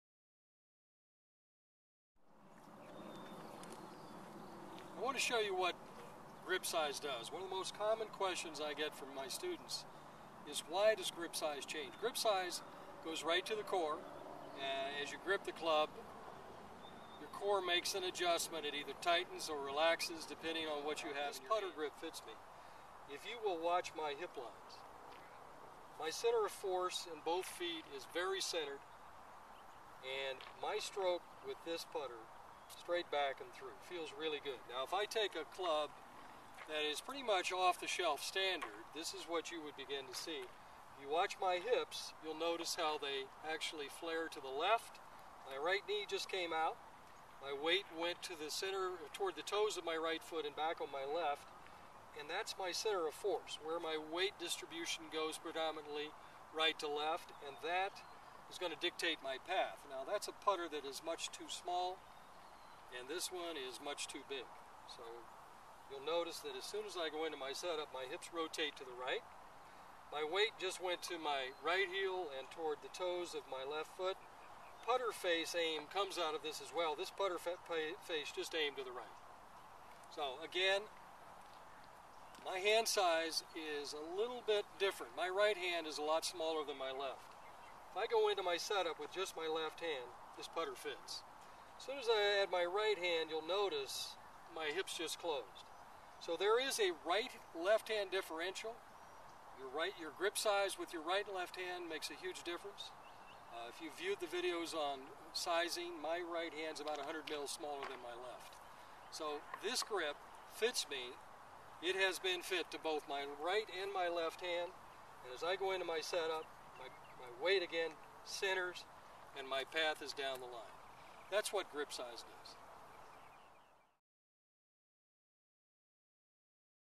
Why Putter Grip Size is Important gripsize.com
Dr. David Wright demonstrates how changes in grip size impact the core muscles causing hip rotation, changes in balance, club face aim and putter path,